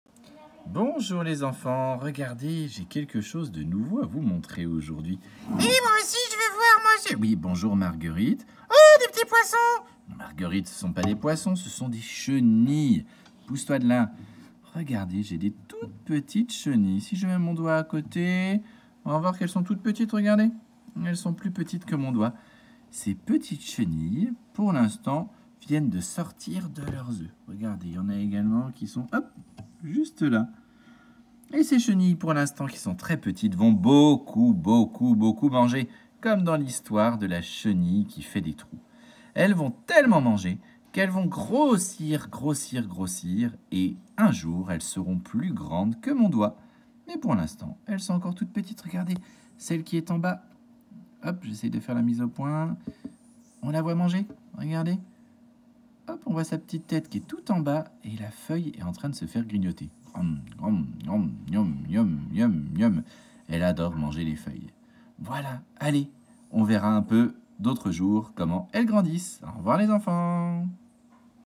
Croissance des chenilles (J 4)
Observation des chenilles du sphinx du frangipanier 4 jours après leur éclosion.